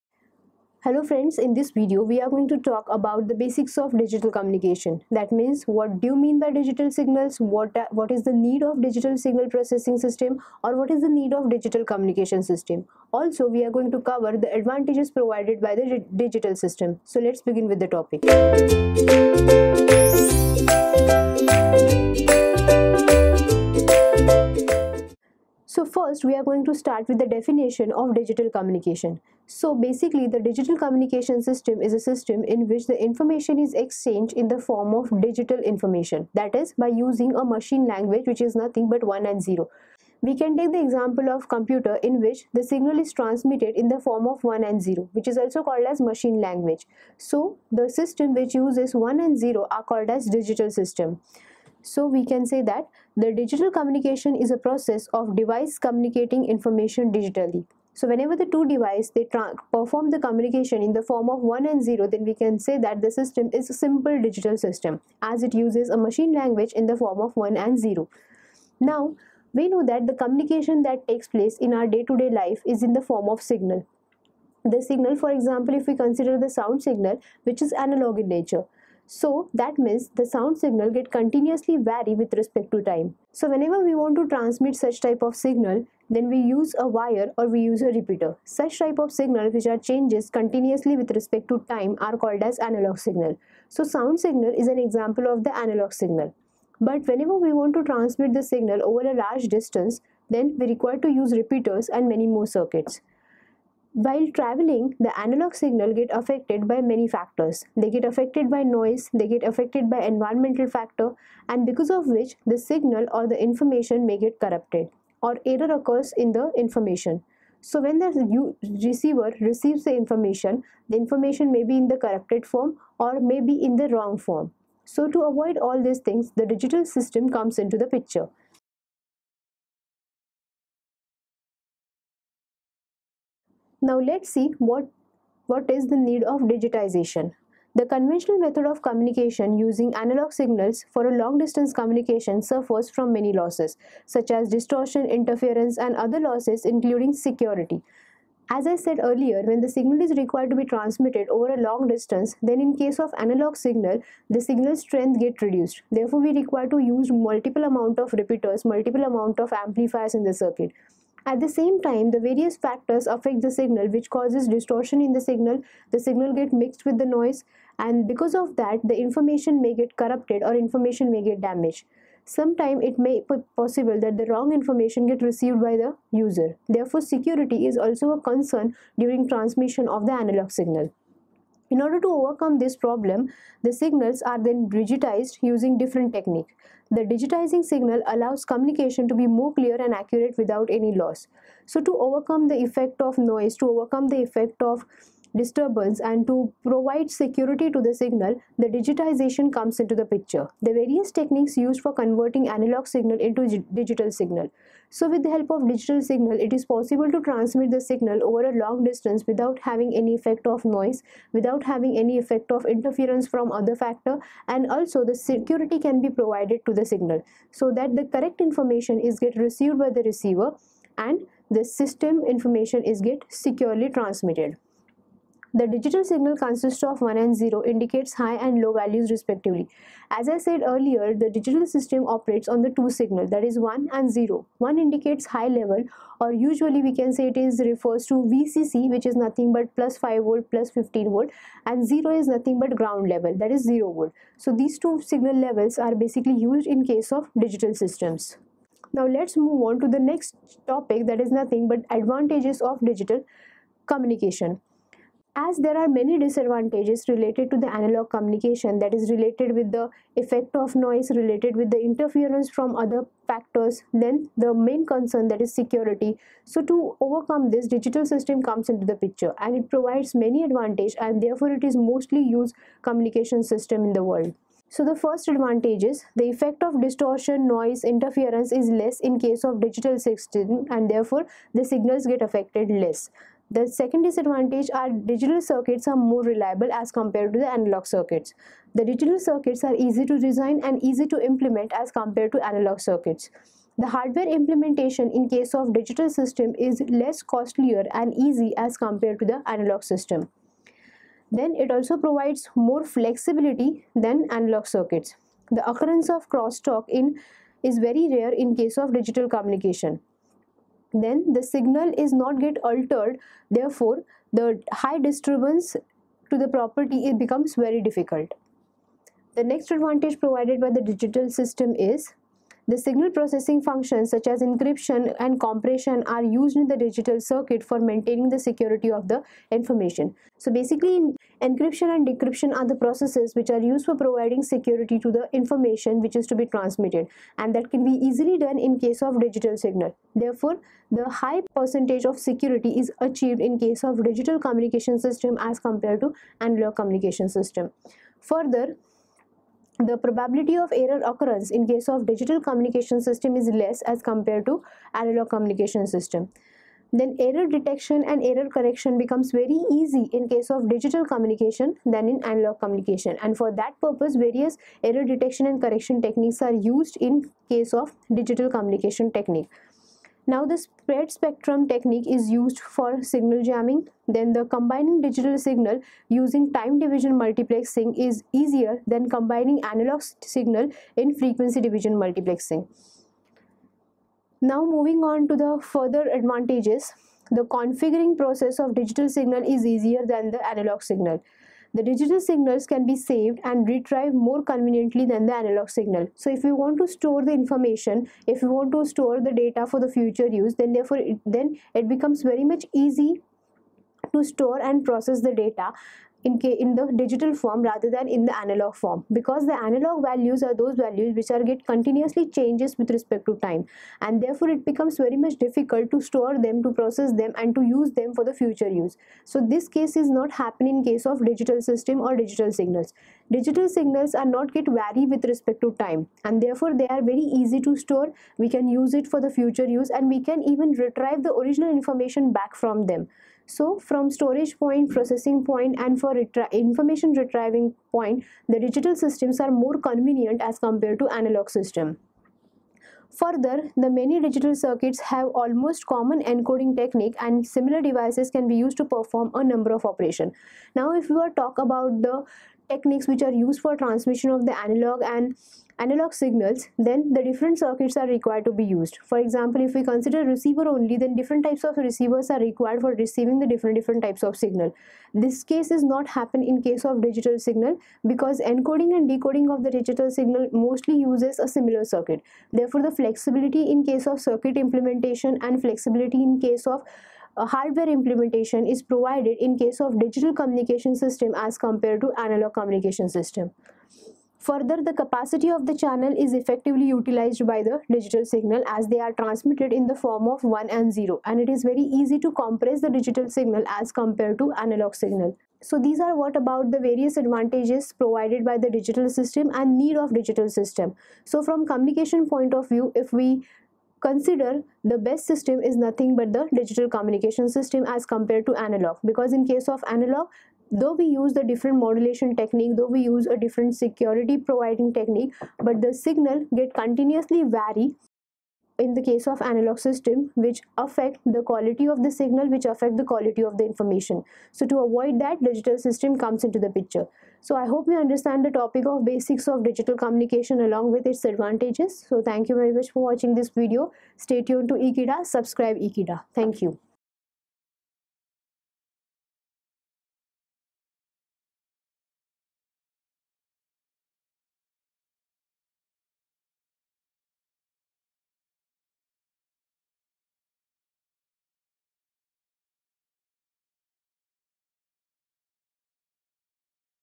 Basics of Digital Communication | TV and Video Engineering
Explore the fundamentals of Digital Communication in TV and Video Engineering! This video delves into the core concepts, covering modulation, encoding, transmission, and more. Discover the essence of conveying data digitally, unraveling the intricacies behind transmitting information effectively. Whether you're a beginner or seeking a refresher, join us to grasp the essential principles of Digital Communication. Dive into this comprehensive guide that simplifies the complexities of transmitting information through digital channels, perfect for enthusiasts and students alike. Master the basics and enhance your understanding of Digital Communication within the realm of TV and Video Engineering!

Happy Learning.